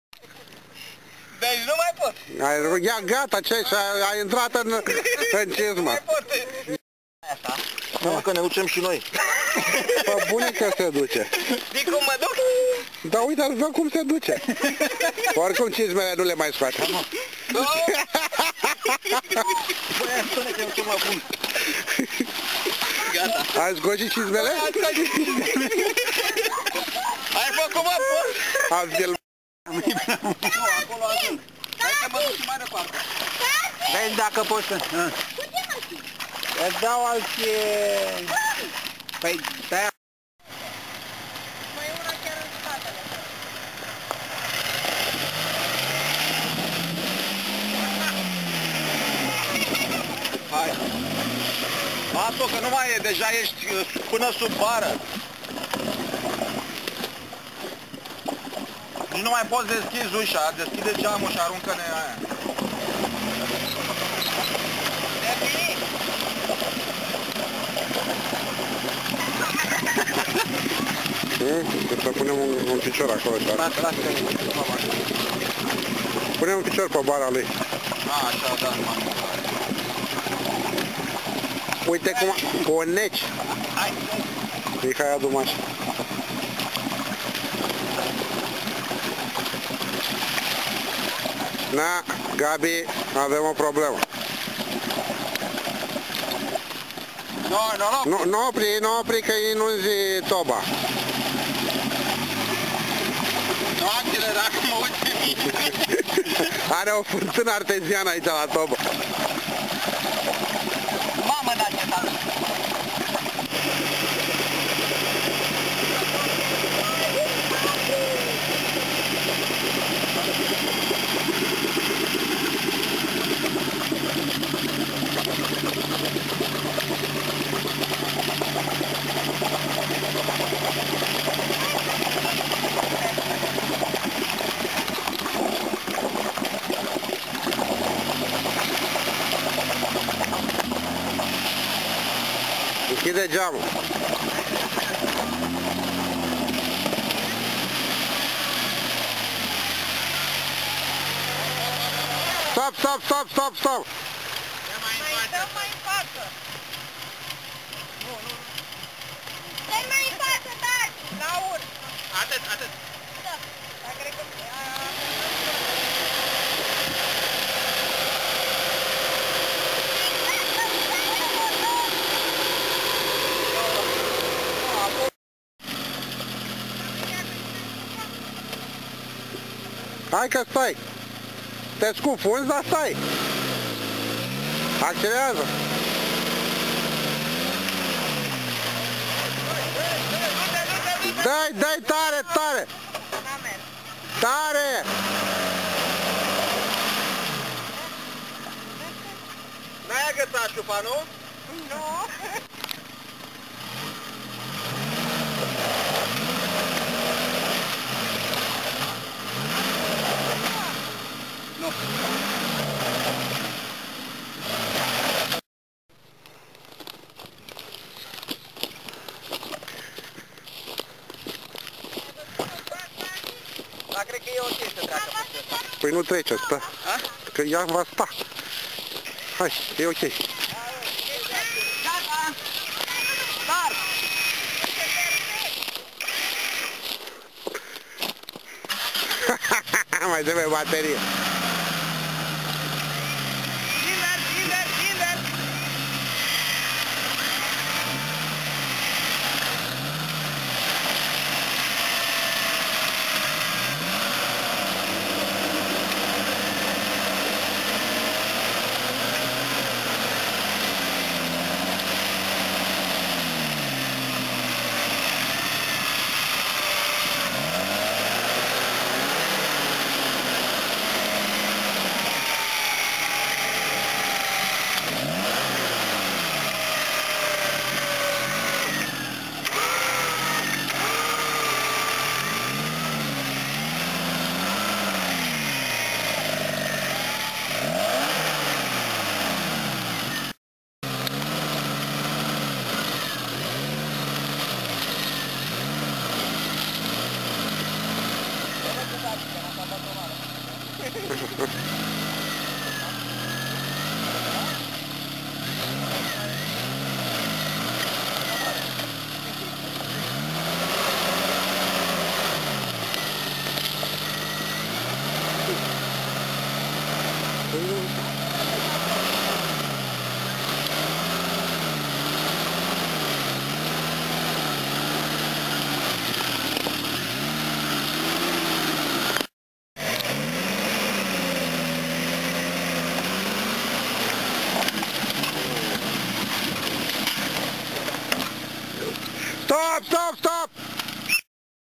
Spre Manastire 21.11.2009
Spre Manastire ... pentru iertarea tuturor pacatelor ... pentru Niva&Samurai!
Am plecat la plimbare cu o misiune ... sa trecem lacul ... si unul dintre noi l-a trecut!